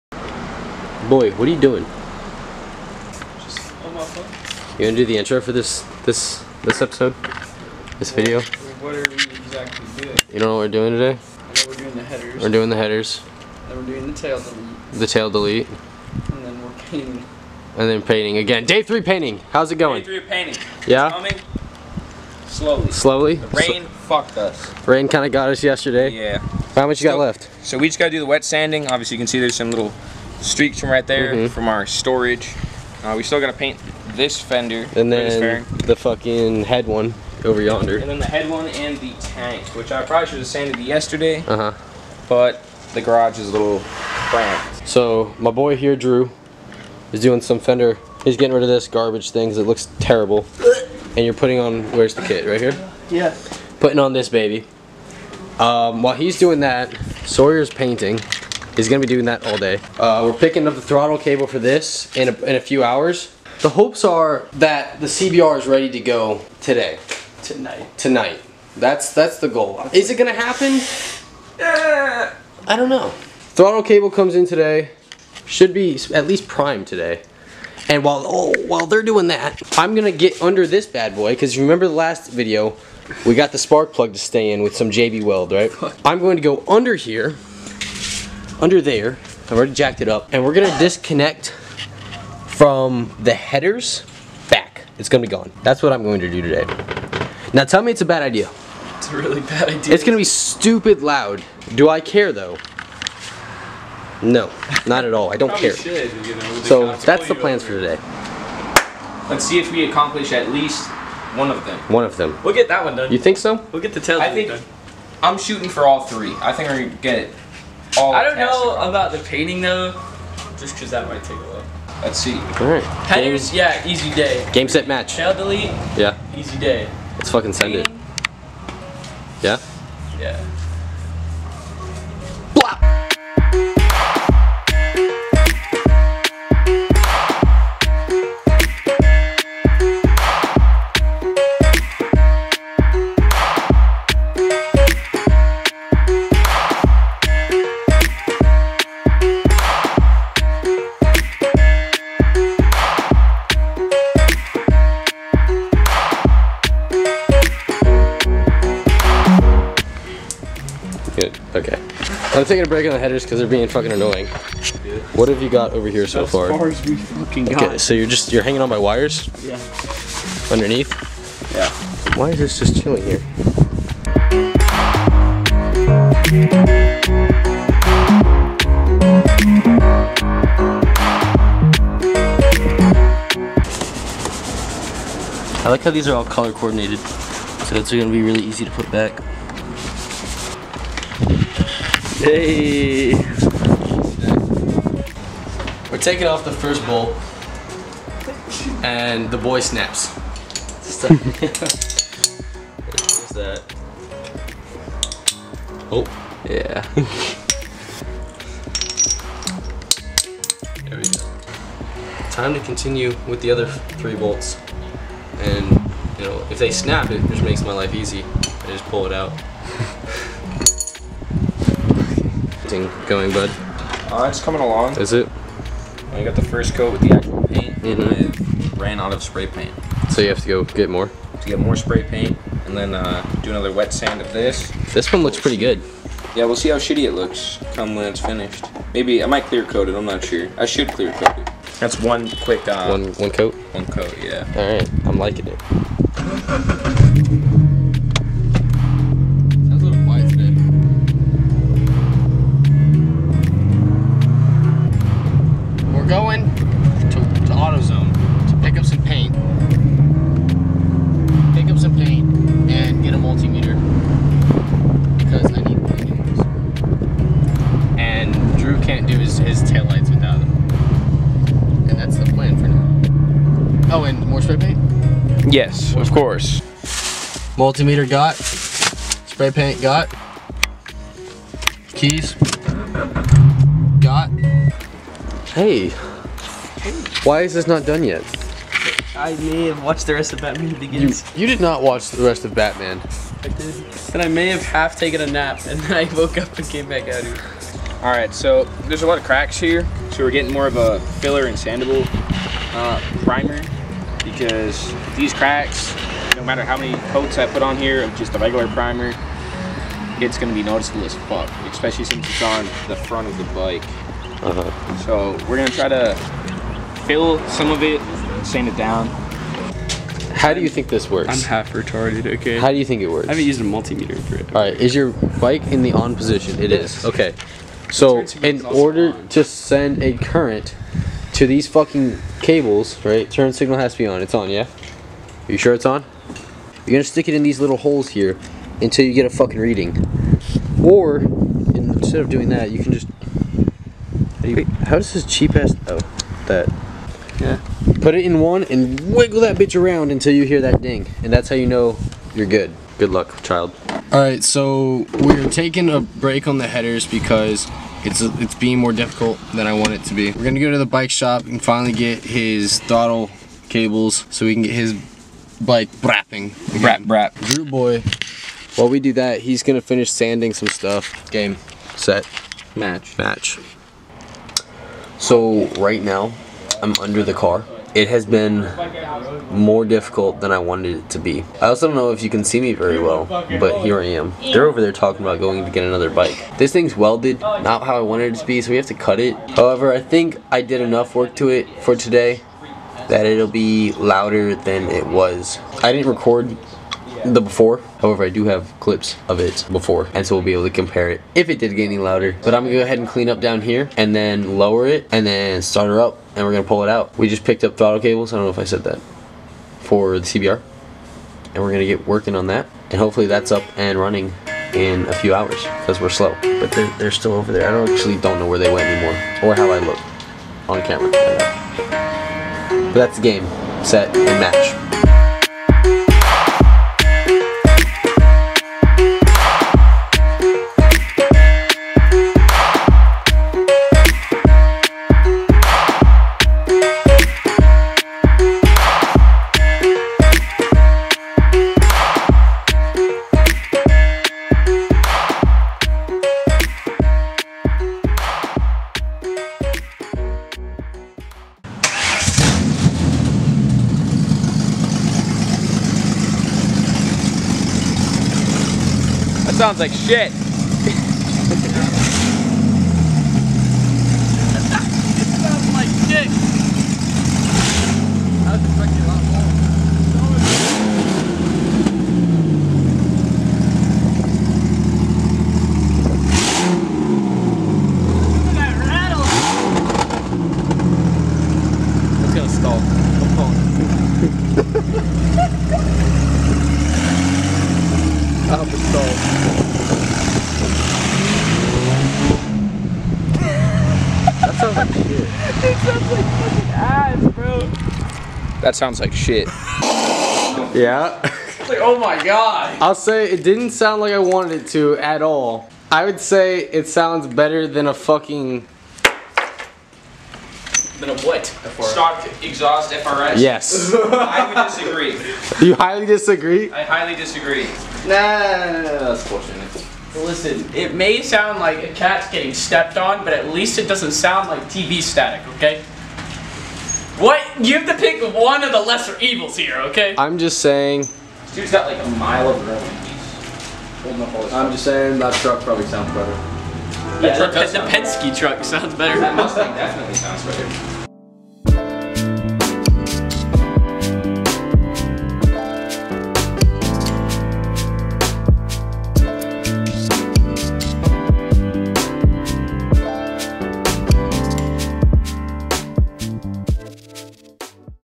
Removing the exhaust on my V6 mustang was a TERRIBLE idea
Leave a COMMENT about this amazing video and what you want to see next.